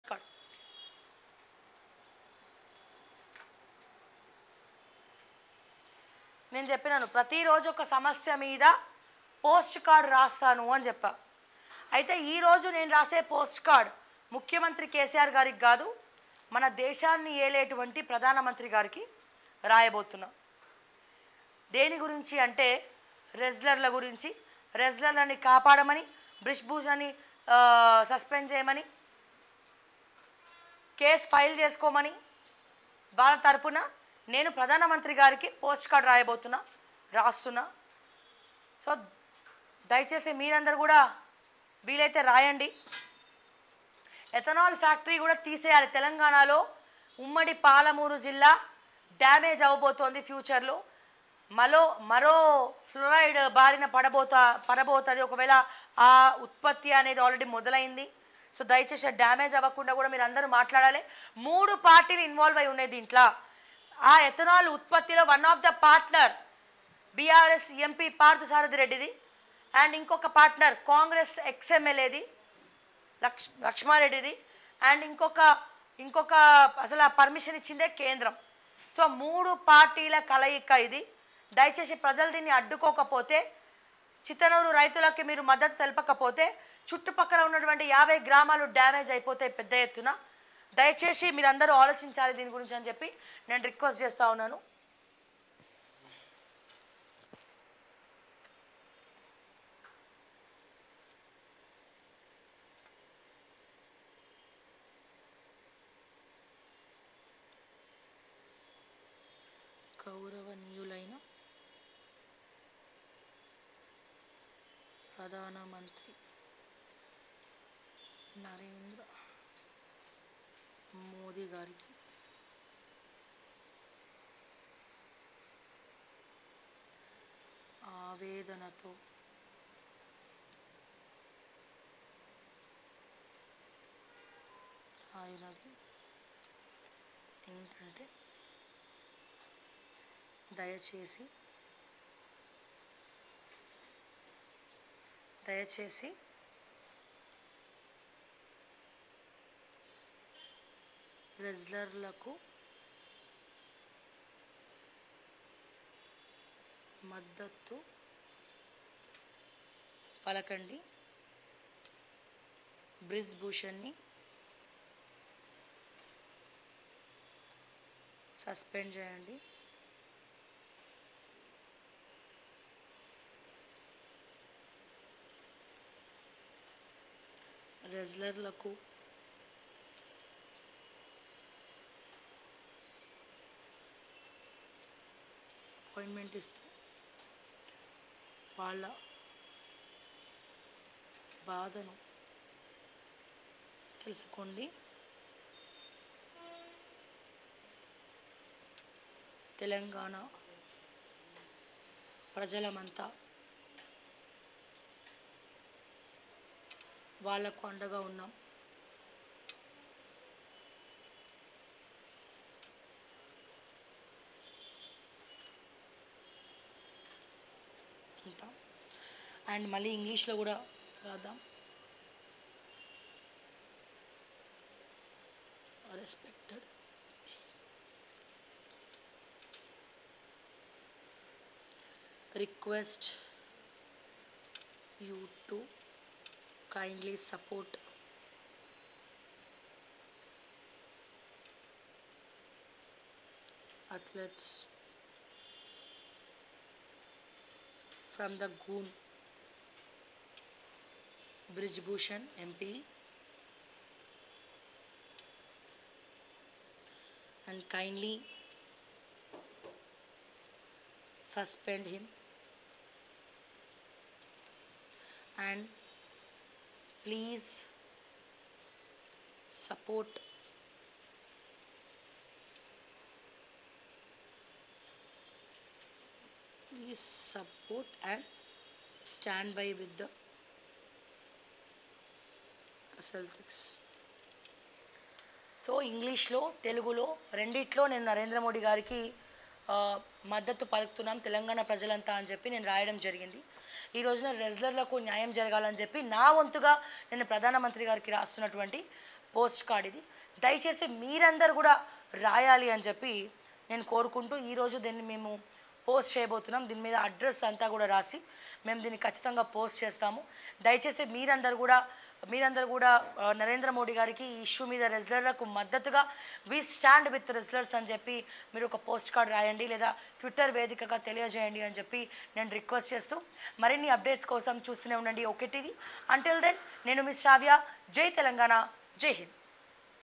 రెజ్లర్ల ఆందోళన పై మోడీ కి శ్రావ్య లేఖ|Journalist Sravya Letter To Modi On Wrestlers Protest | OK TV
రెజ్లర్ల ఆందోళన పై మోడీ కి శ్రావ్య లేఖ  | Journalist Sravya Letter To PM Modi On Wrestlers Protest | OK TV